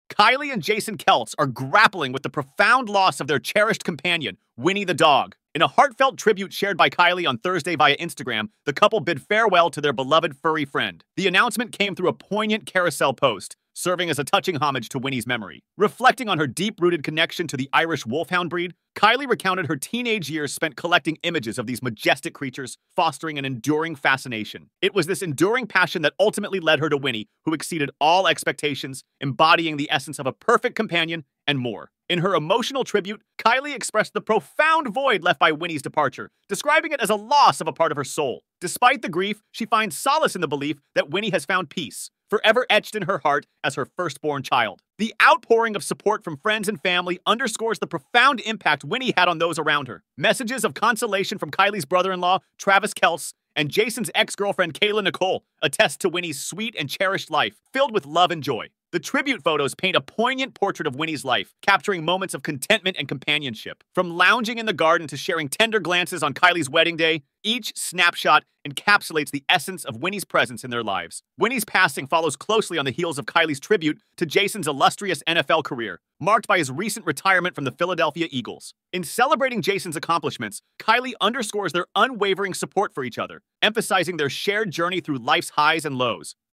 Kylie and Jason Kelce Mourn Death of Their Dog Winnie
Join us as we pay tribute to the cherished life of Winnie, the beloved Irish Wolfhound companion of Kylie and Jason Kelce. In this heartfelt video, we share the touching carousel post from Kylie's Instagram, commemorating Winnie's profound impact on their lives. Through tender moments captured in photos, we celebrate Winnie's joyful presence and the enduring love shared between a pet and their family. From garden lounging to wedding day glances, every snapshot embodies the essence of Winnie's companionship. As we honor Winnie's memory, we also reflect on Jason Kelce's illustrious NFL career and recent retirement from the Philadelphia Eagles, showcasing the bond of love and support between Kylie and Jason. Join us in remembering Winnie and celebrating the profound joy she brought to all who knew her.